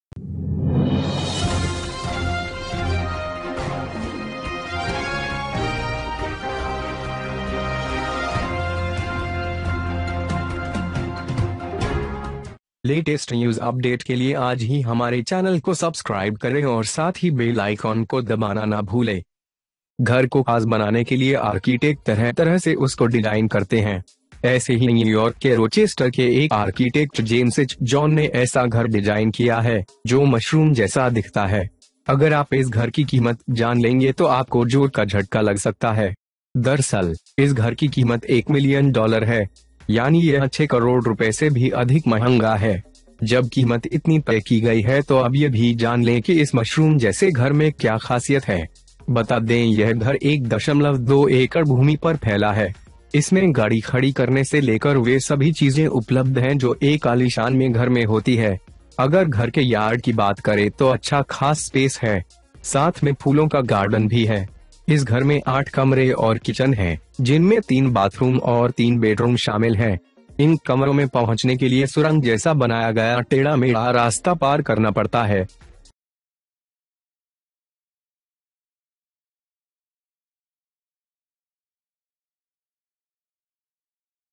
मशरूम जैसा ये घर देखकर कहीं आंखें न चौंधिया जाए, कीमत भी आपकी सोच के परे
Architect designs the way to make the house special. Similarly, James H. John, an architect of Rochester in New York, designed a house that looks like a mushroom. If you know the value of this house then you may feel a shock of loudness. Actually, the price of this house is $ 1 million. That is, it is more expensive than 6 crores. When the price has been fixed, now also know what is the specialty in the house like this mushroom. Tell this house is spread over 1.2 acres of land. There are all the things available in the house in a luxurious house, from caring in it. If you talk about the yard of the house then there is a special space. There is also a flower garden as well. This house has 8 rooms and kitchens, which include 3 bathrooms and 3 bedrooms. In order to reach these rooms, there is a crossroads made in the tunnel like the tunnel.

BB NEWS!!